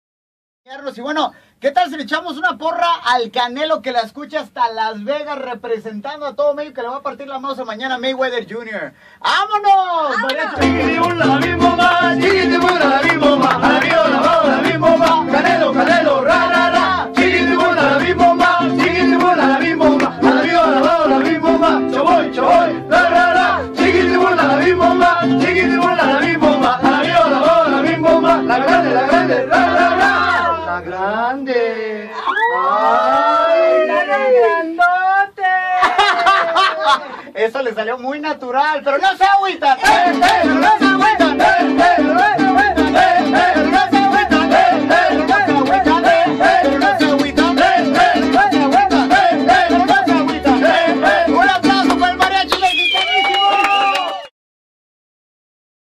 Independencia De Mexico Con Shoboy En La Manana
El Mariachi Mexicanisimo, Carolina Imperial y Javier Aguilar fueron parte de la celebracion de Shoboy en La Mañana por la Independencia de Mexico!